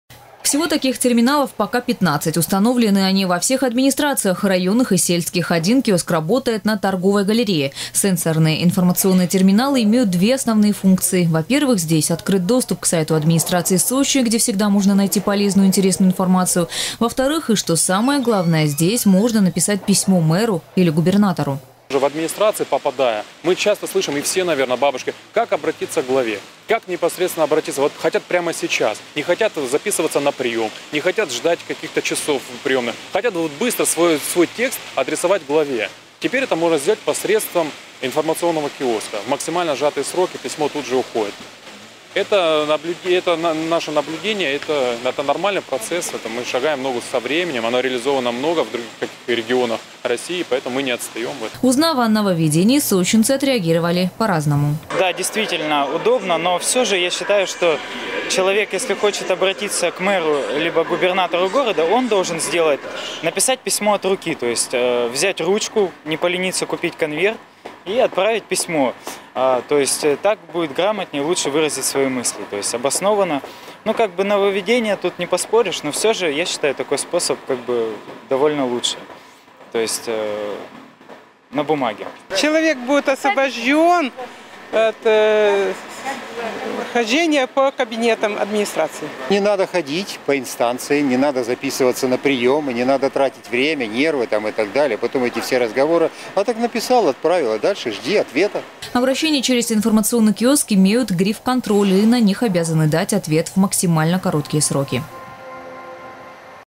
В Сочи заработали информационные киоски. Новости 24 Сочи
Сенсорные терминалы открывают для сочинцев доступ к правительству. По средством такого соверменного оборудования, всё желающие могут отправить электронное письмо главе города Сочи или губернатору Краснодарского края.
Сочи. Новости.
Телеканал Эфкате.